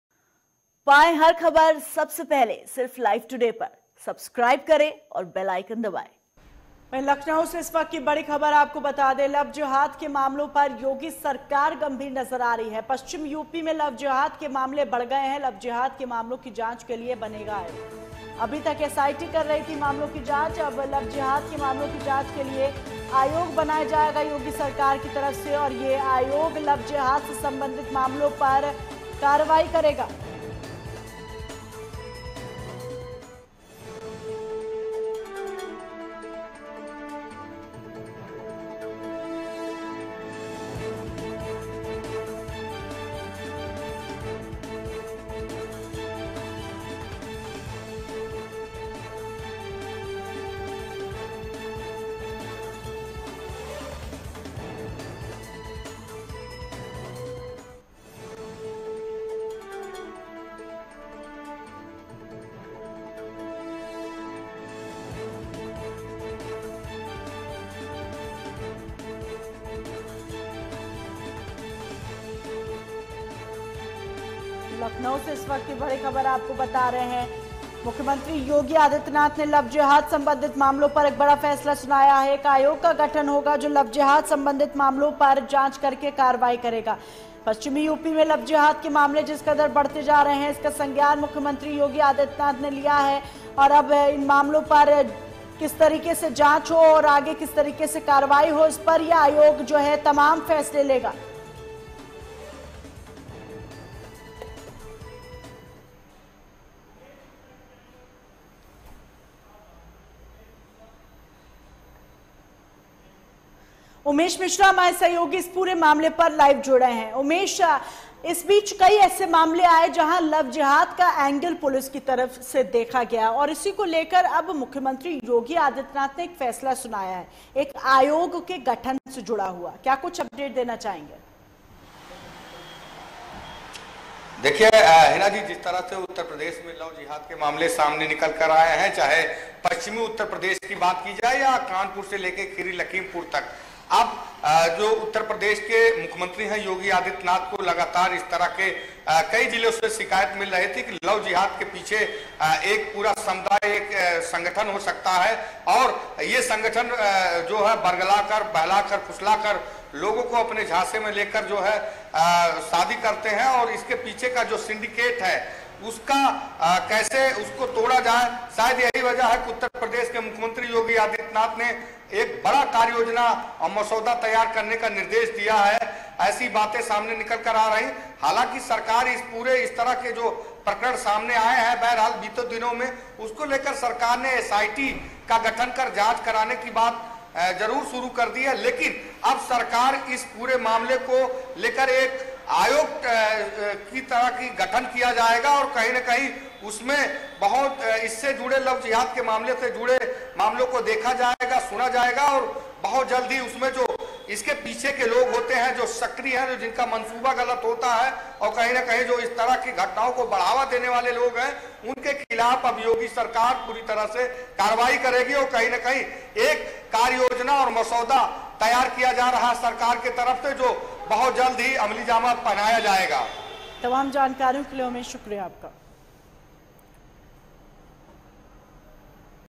पश्चिमी यूपी में बढ़ते लव जेहाद के मामलों पर सीएम योगी सख्त, बनेगा आयोग |religious conversion
हमसे जुड़े रहने के लिए हमें फॉलो करें